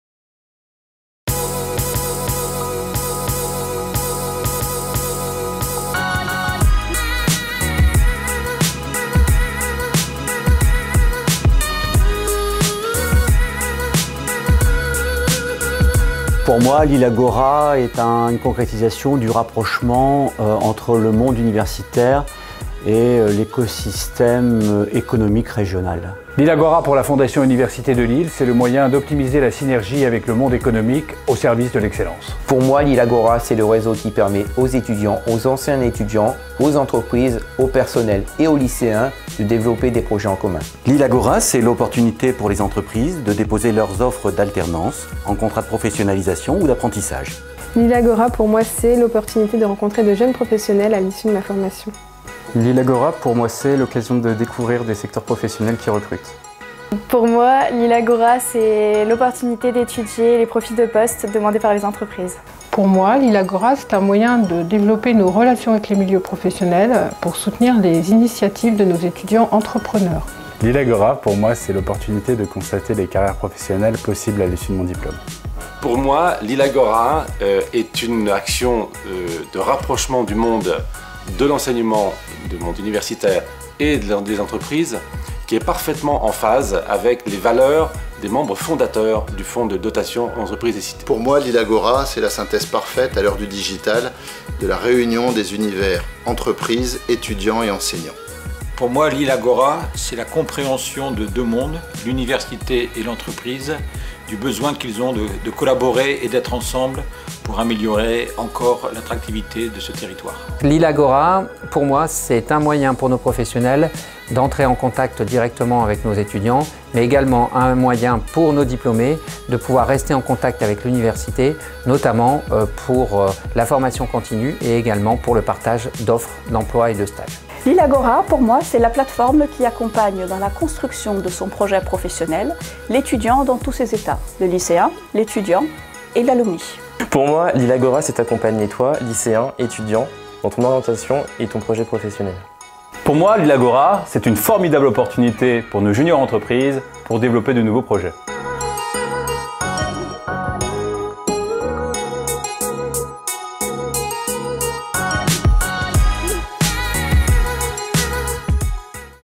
Lilagora c'est quoi ? vidéo 2
### Lilagora, le réseau professionnel de l'Université de Lille ###

Grâce à cette interface, véritable lieu d'échanges et d'interactivité sur le web, les cinq publics membre de ce nouveau réseau professionnel (étudiants, alumni, partenaires, personnels et lycéens) pourront en un seul lieu :

-Proposer et saisir des opportunités professionnelles
- Lancer et approfondir des partenariats ou des collaborations, notamment en matière de recherche et de formation
- acquérir de nouvelles compétences
- parrainer ou devenir mentor d'un étudiant ou lycéen...

Et ainsi développer un lien durable et profitable à chacun.